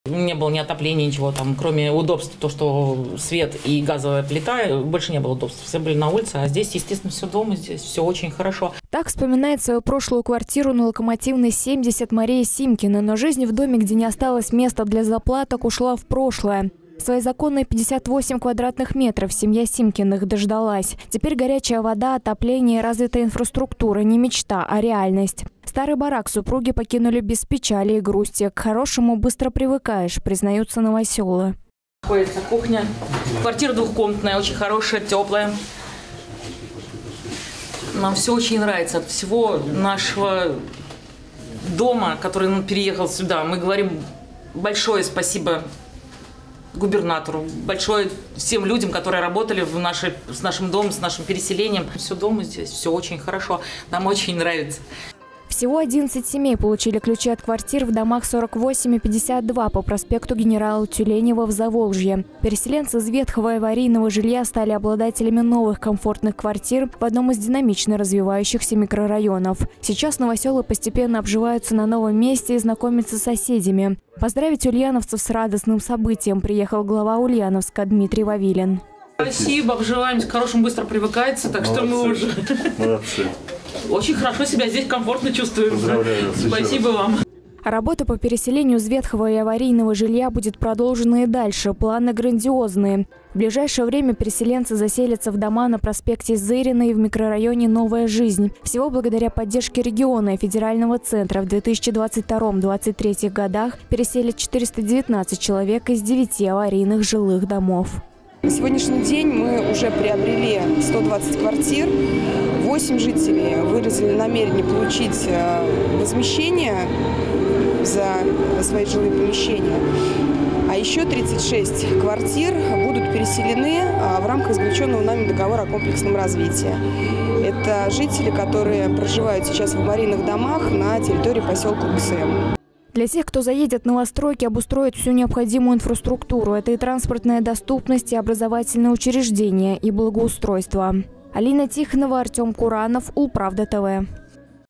В Ульяновске переселенцам из ветхого жилья вручили ключи от новых квартир
В Ульяновской области реализуется программа реновации жилья. На очереди десятки новых домов и сотни жителей. Глава Ульяновска Дмитрий Вавилин встретился с горожанами, которые в этом году переехали из аварийного жилья в новые дома №48 и 52 по проспекту Генерала Тюленева.